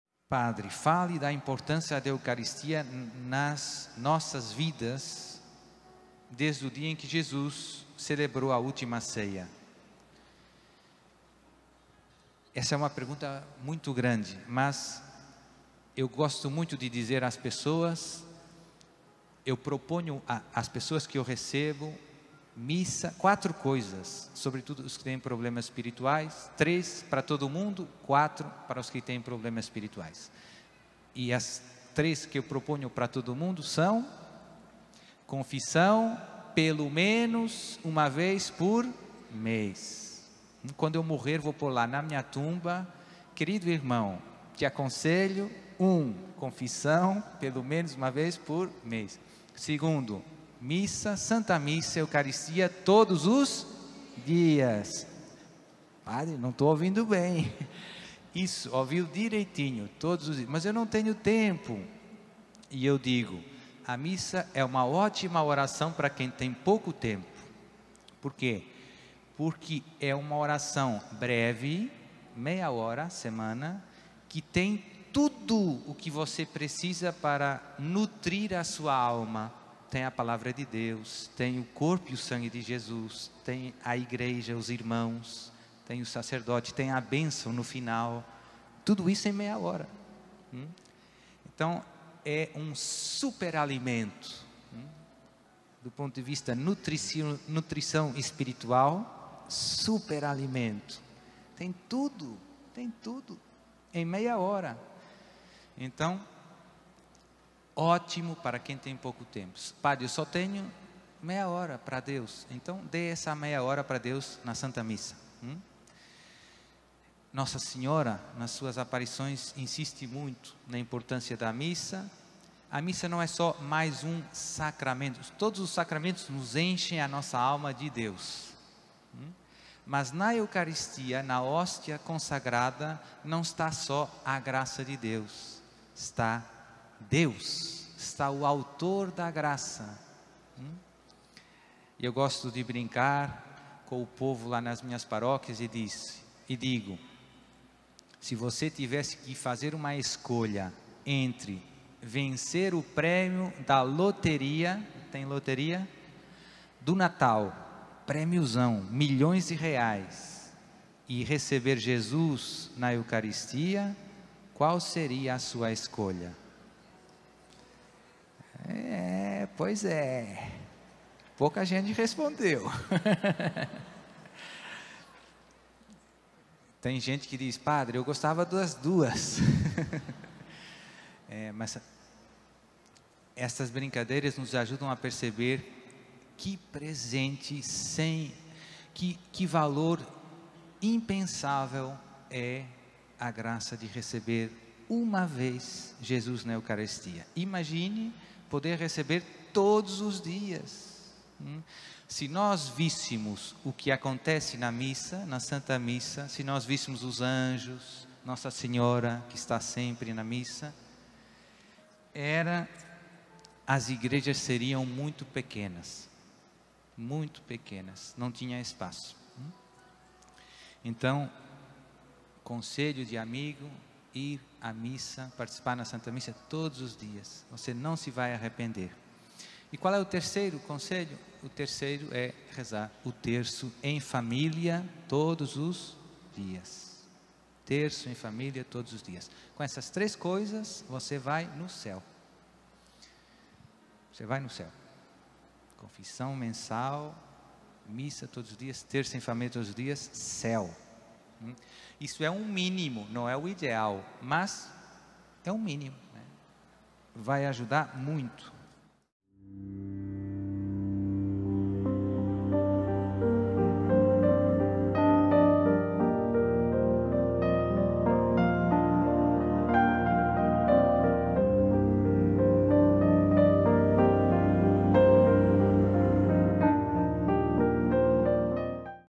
O MÍNIMO PARA ALCANÇAR O CÉU - PADRE DUARTE LARA
Neste vídeo, o Padre Duarte Lara fala sobre a importância de estarmos sempre em comunhão com a igreja para mantermos nossa alma sempre no céu.